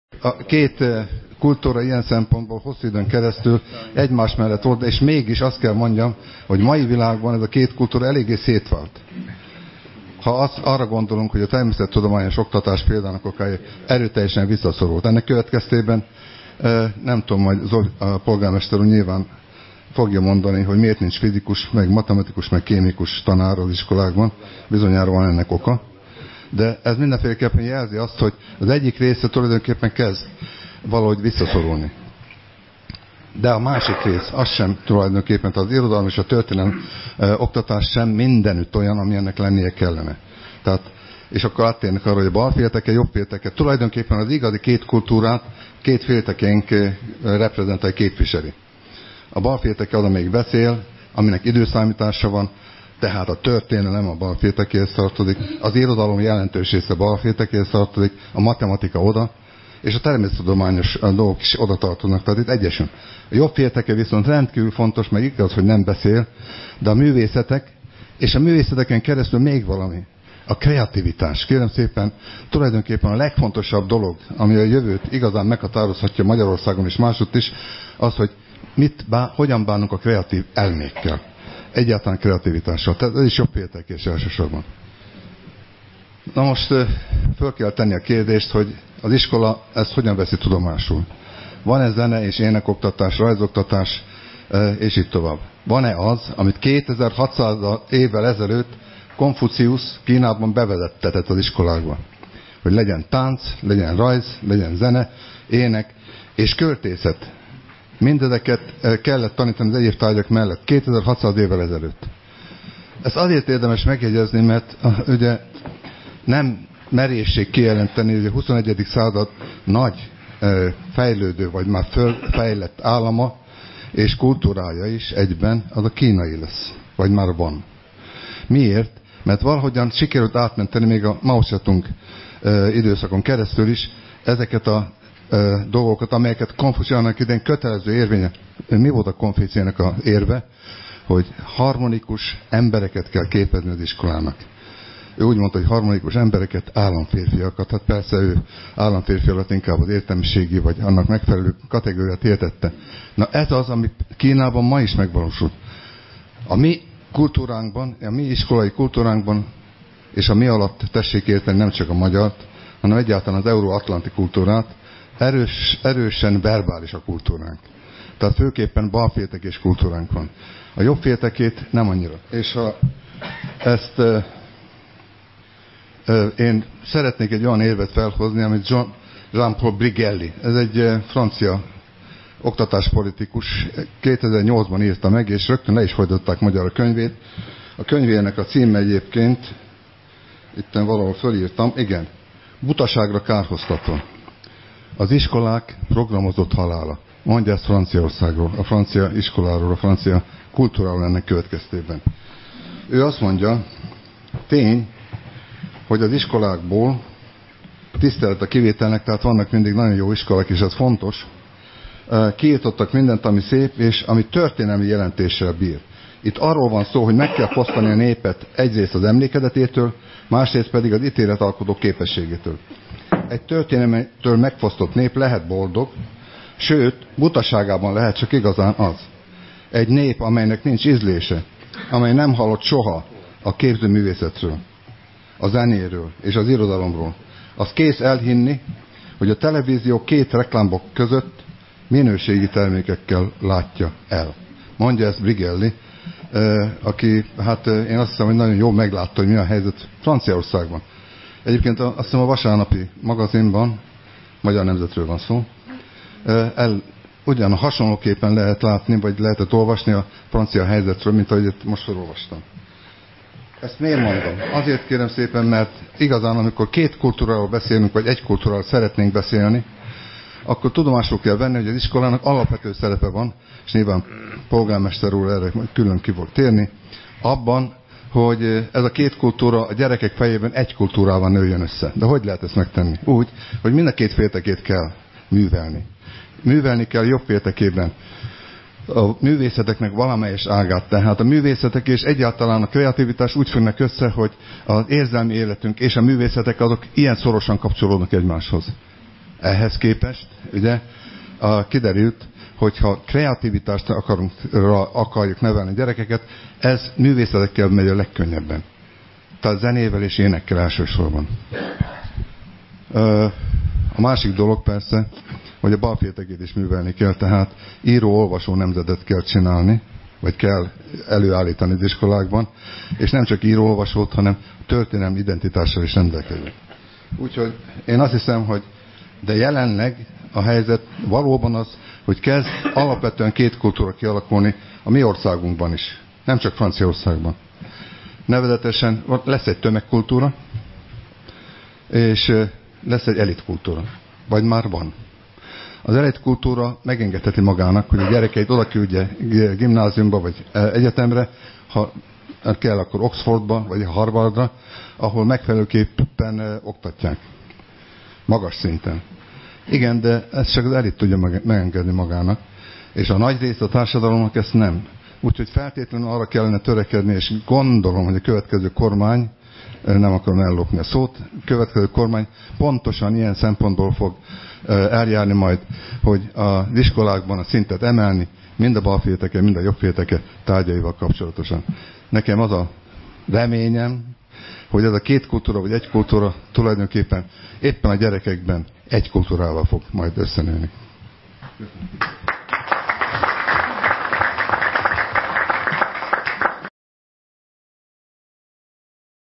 04 _Baross Gábor Társaság 2009. 11. 25,
A Baross Gábor Társaság ez évi utolsó összejövetele„Két kultúra egy kultúra címmel. Dr. Hámori József akadémikus,  volt miniszter, egyetemi tanár vitaindító előadása.